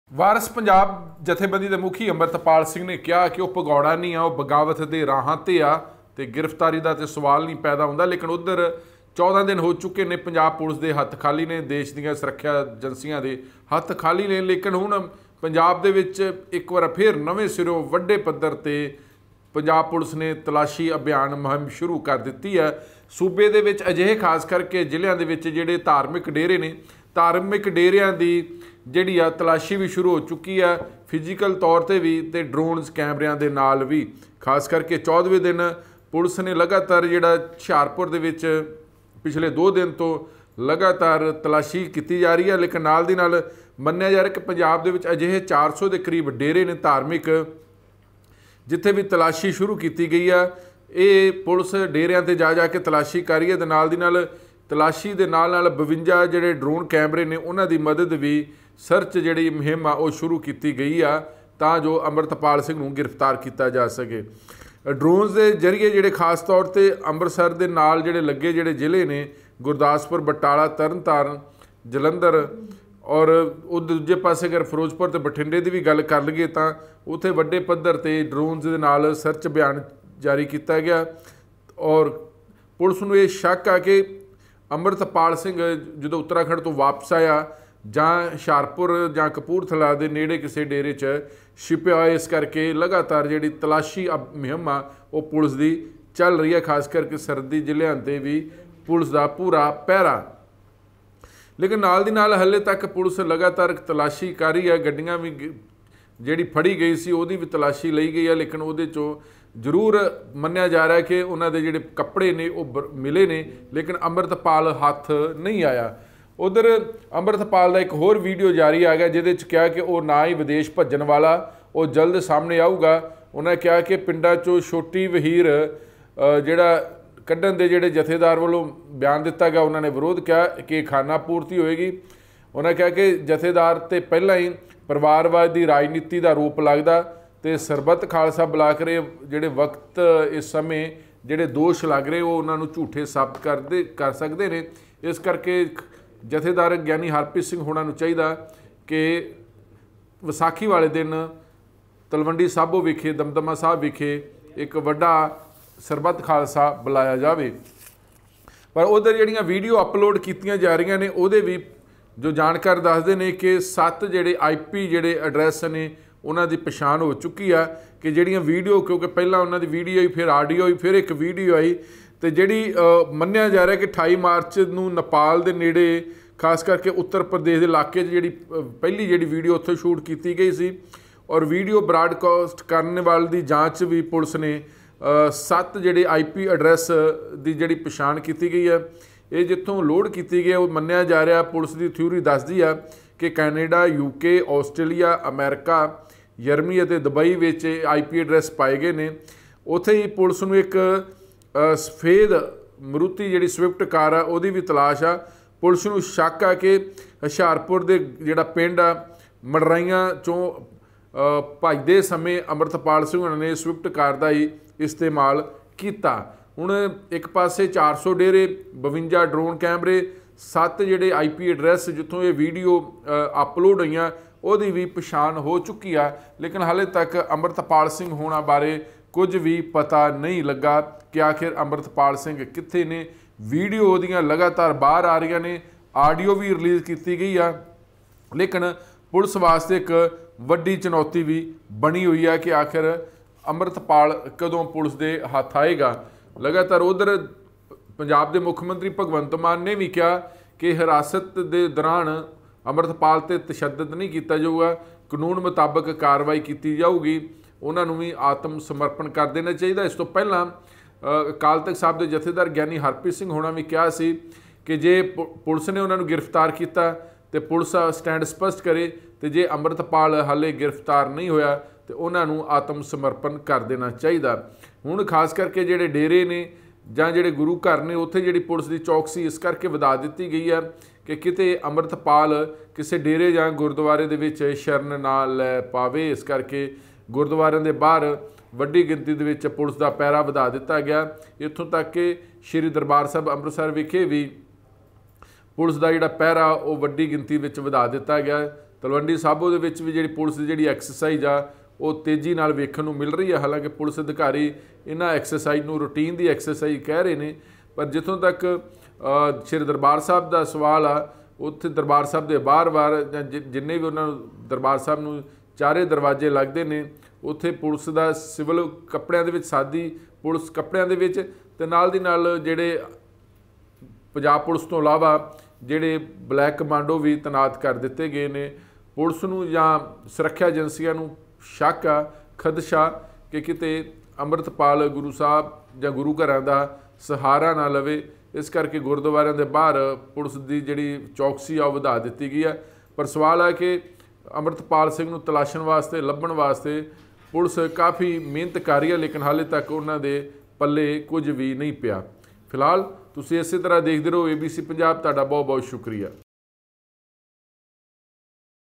ਅਪਰੇਸ਼ਨ AP ਦੀ ਭਾਲ ਲਈ ਸੂਬੇ ਭਰ ਦੇ ਕਈ ਡੇਰਿਆਂ ਦੀ ਤਲਾਸ਼ੀ
ਡਰੋਨ ਕੈਮਰਿਆਂ ਦਾ ਸਹਾਰਾ, ਗੁਰੂ ਘਰਾਂ ਦੇ ਬਾਹਰ ਪੁਲਿਸ ਦਾ ਪਹਿਰਾ ਸਖ਼ਤ
ਵੀਡੀਓ ਕਿਥੋਂ ਅਪਲੋਡ ਹੋਈ, ਪੁਲਿਸ ਨੂੰ ਮਿਲੇ ਪਤੇ